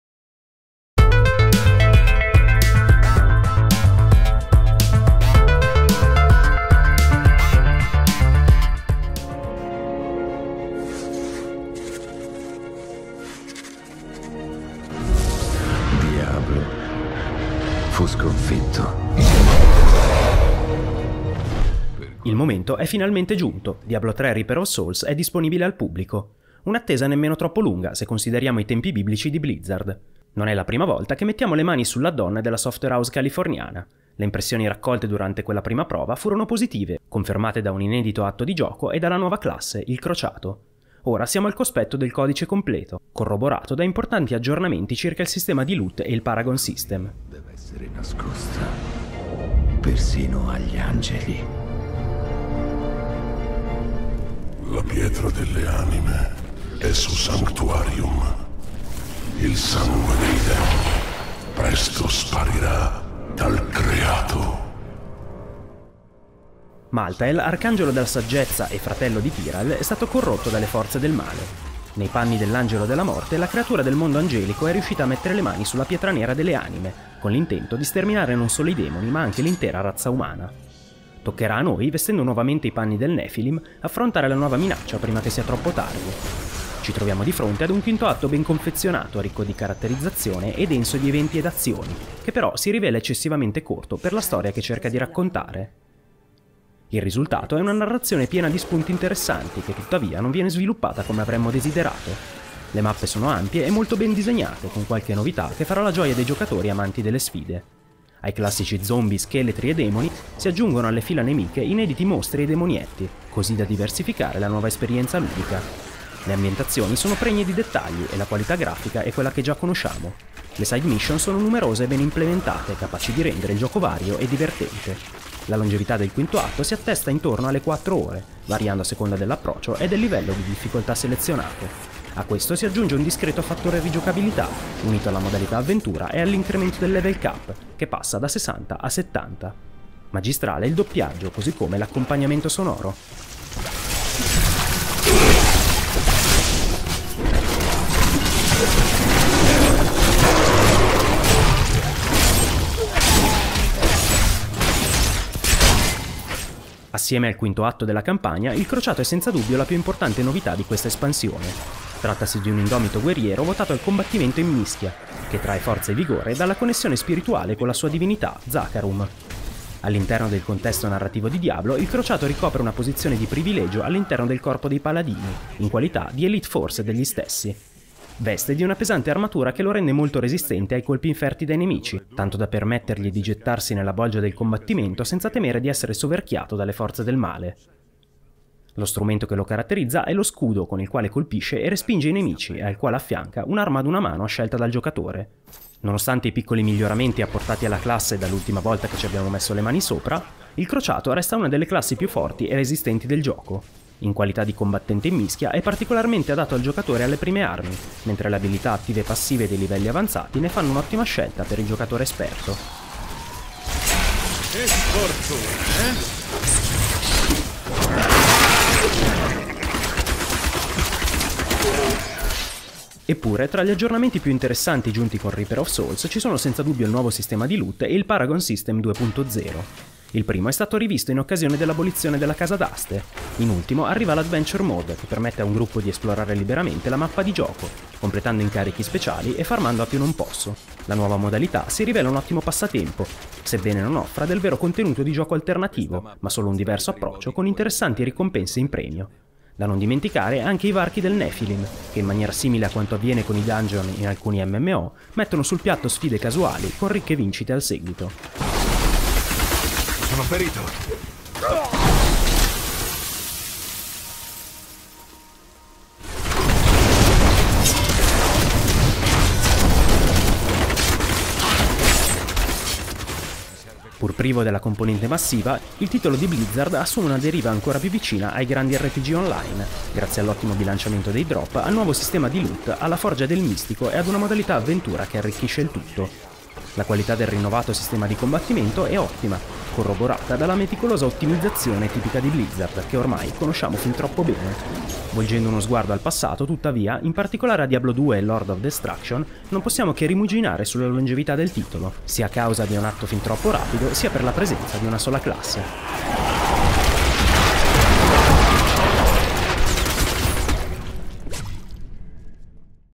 Diablo 3: Reaper of Souls - Video Recensione ITA HD - Everyeye.it
Disponibile la Videorecensione di Diablo 3: Reaper of Souls, espansione dell'ultimo hack'n'slash made in Blizzard. Nei panni del Crociato o di uno dei vecchi personaggi di gioco, potremo affrontare un inedito quinto atto della campagna, sventando i malefici piani di Malthael, l'Angelo della Morte.